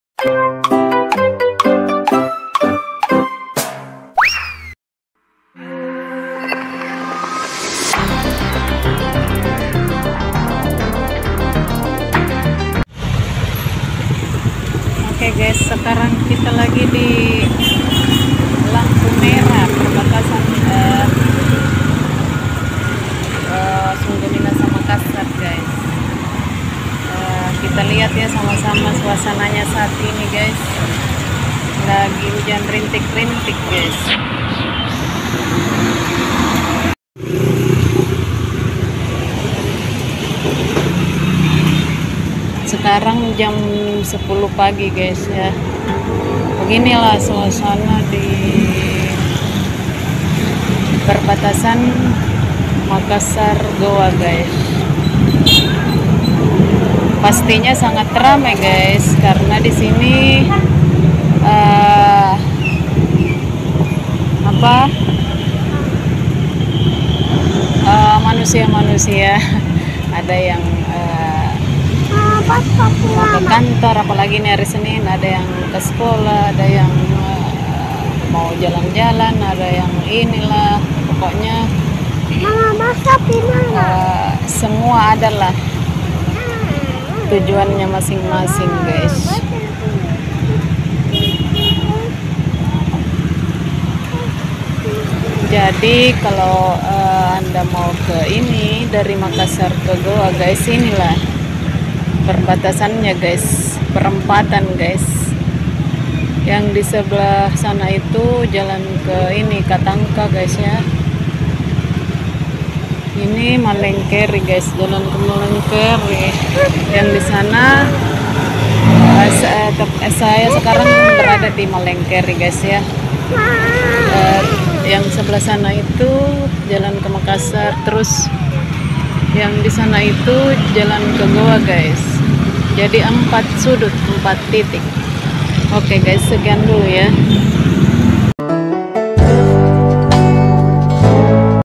Suasana perbatasan Makassar - Gowa, senin pagi, 9 Januari 2023
Inilah hingar bingar suasana perbatasan Makassar - Kab. Gowa diawal tahun 2023